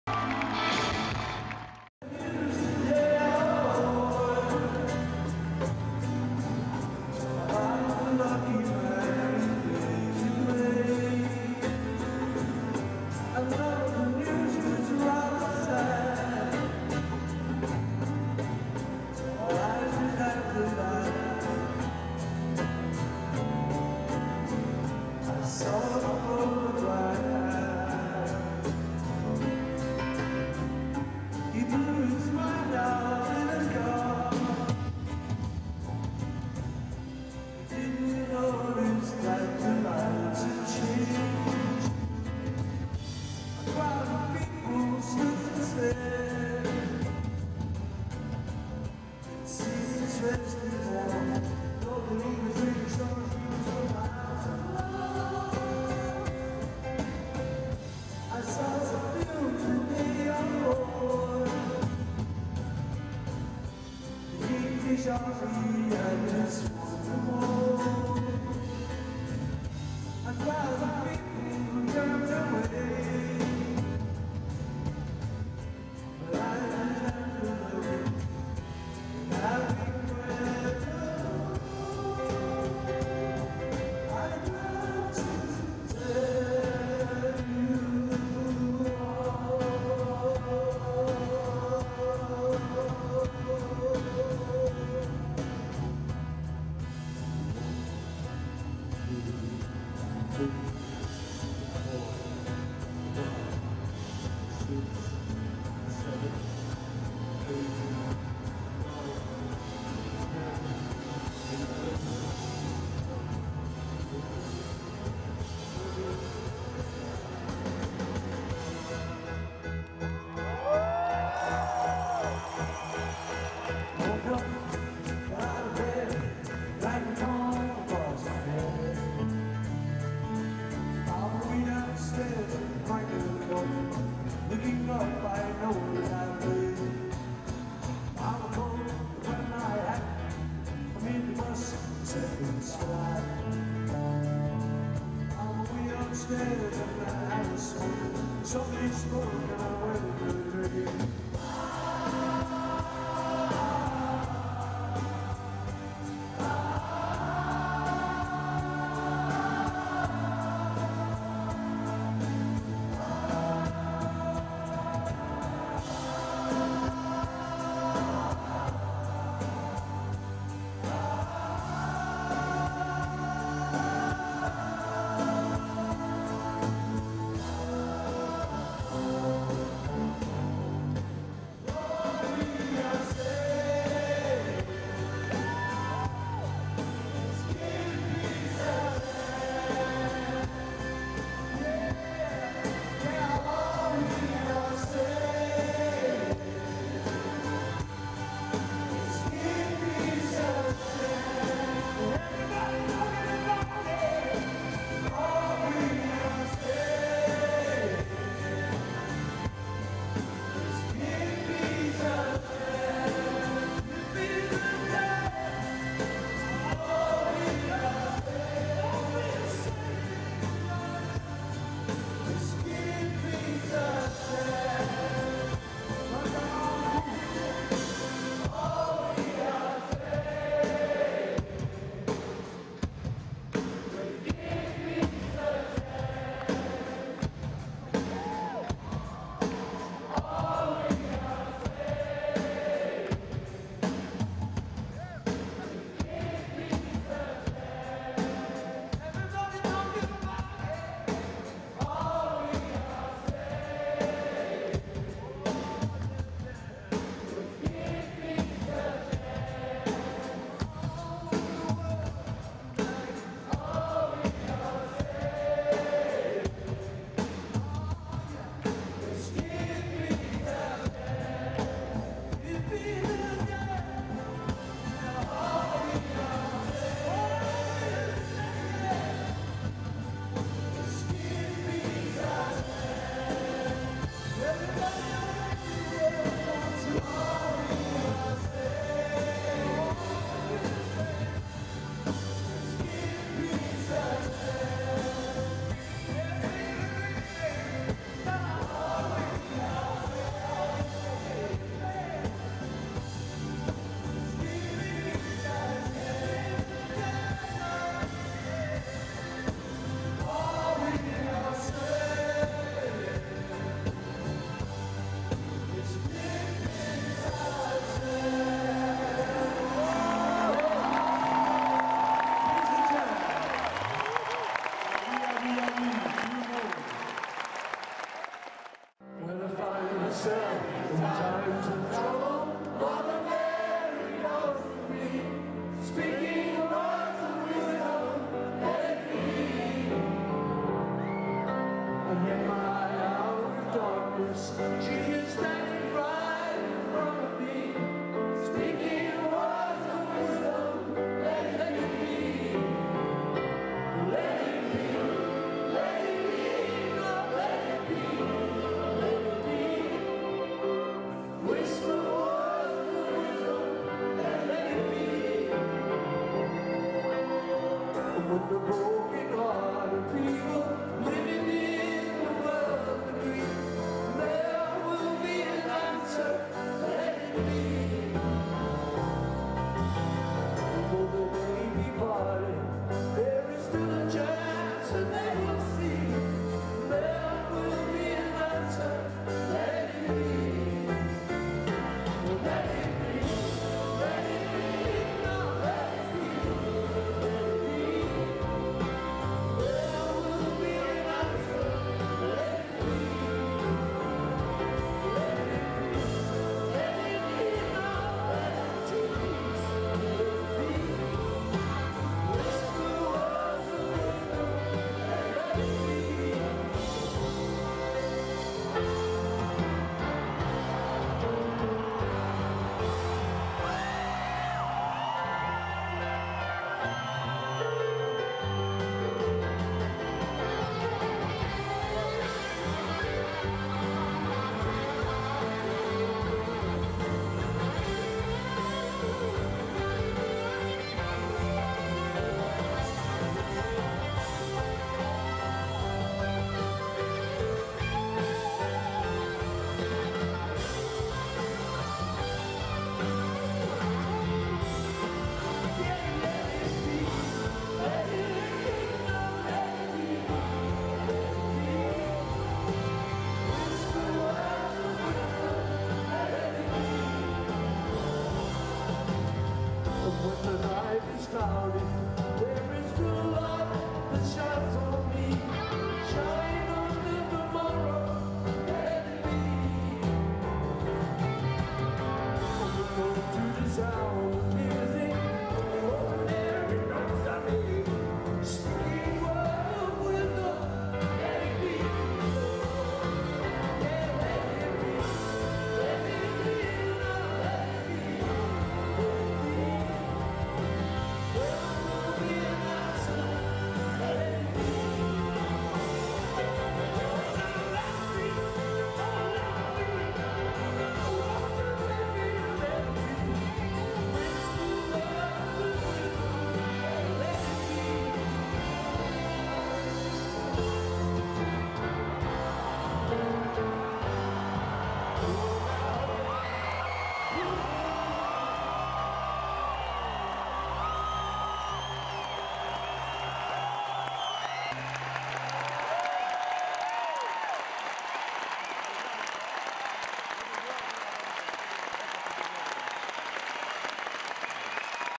Paul McCartney Bercy 2011 part 10/14
Reconstitué à partir de 117 vidéos postées sur YouTube, nous voici à la 10é partie du concert de Paul McCartney donné à Bercy le 30 novembre 2011. A suivre...